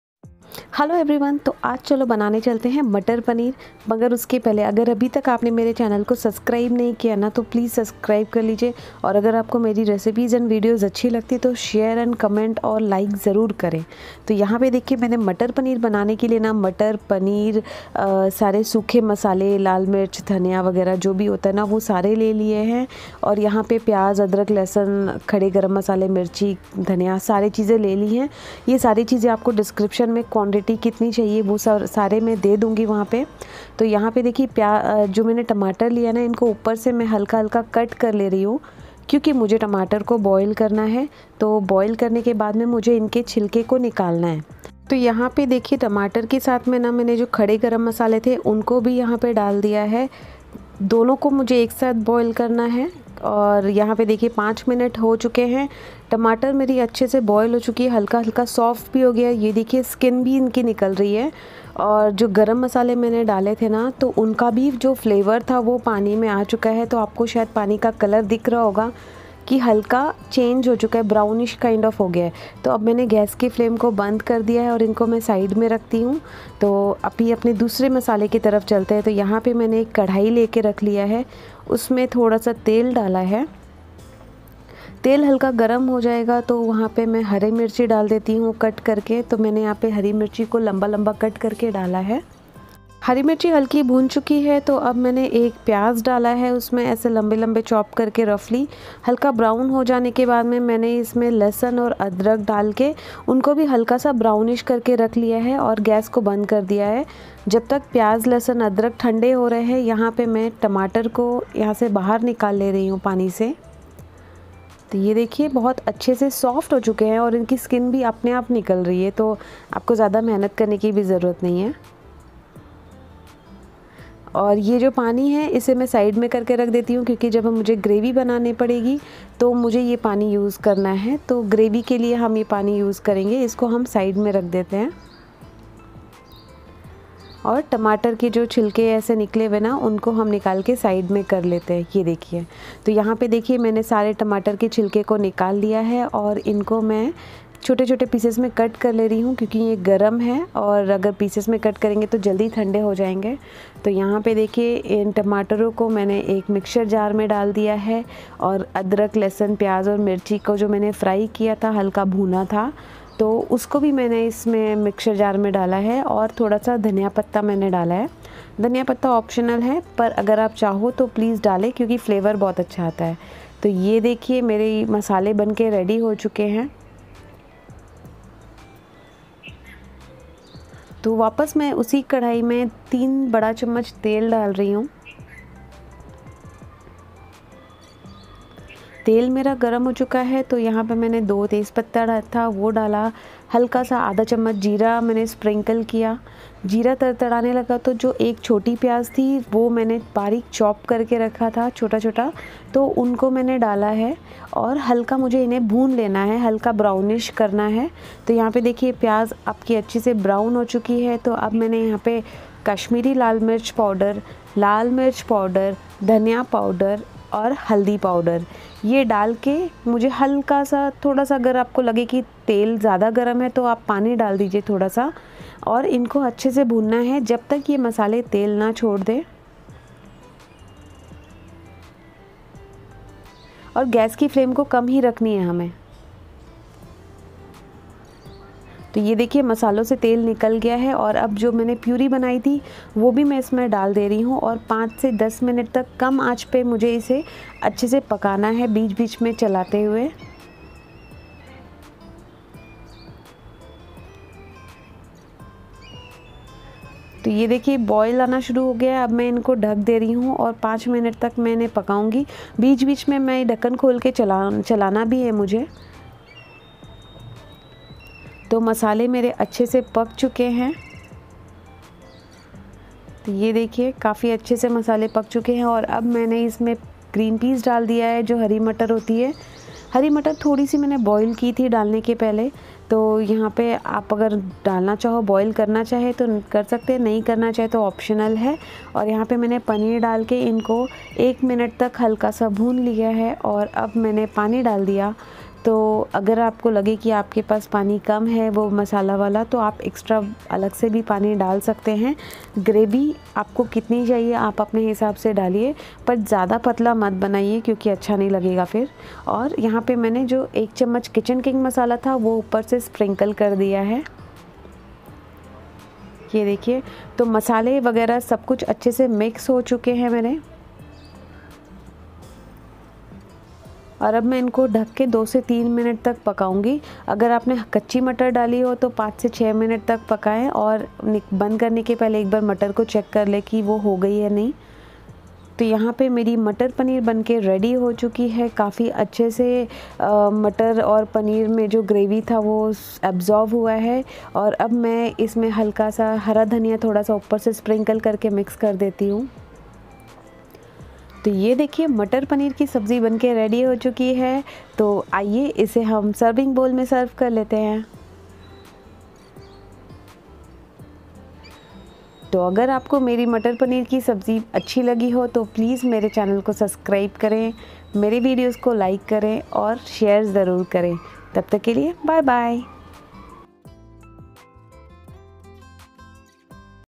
MatarPaneer in unique style Paneer मटर पनीर बनाने का ये तरीका देख के कहेंगे पहले क्यों नहीं पता था
Green Peas- 250 Grams
Paneer - 200 Grams
2 Onions
3 Tomatoes
6-7 garlic
1 inch Ginger
2 Green Chilies
Coriander Leaves

Clover - 3-4
Cinnamon - 1 inch
Green and Black cardamom - 1

Kashmiri Lal Mirchi powder - 1 Table Spoon
Lal Mirchi powder - 1/2 Table Spoon
Coriander Powder - 1 Table Spoon
Turmeric - 1 Tea Spoon
Kitchen King Masala or Garam Masala or Sabjee Masala - 1 Table Spoon